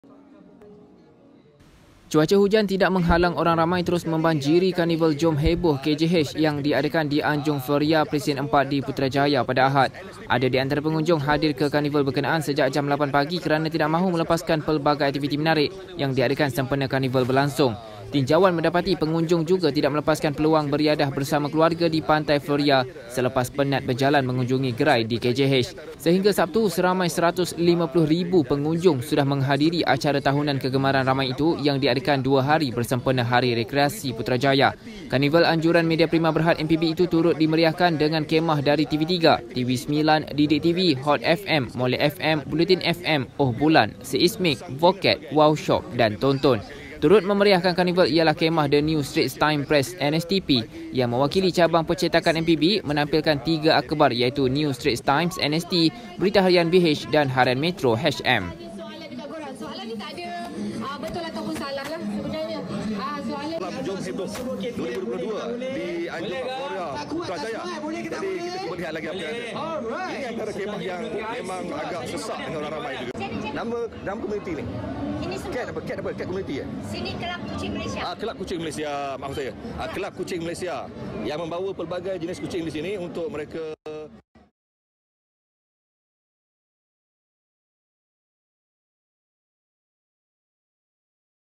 Lebih 150,000 pengunjung datang ke Karnival Jom Heboh
PUTRAJAYA: Cuaca hujan tidak menghalang orang ramai terus membanjiri Karnival Jom Heboh (KJH) yang diadakan di Anjung Floria, Presint 4, di sini, hari ini.